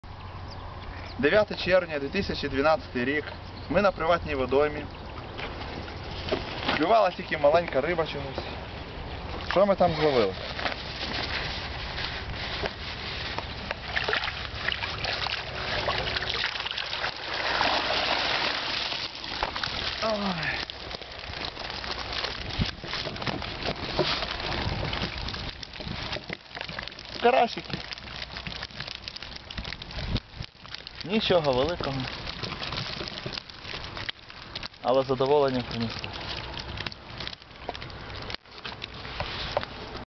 результат рибалки 9 червня 2012
Все зловлено одним пікером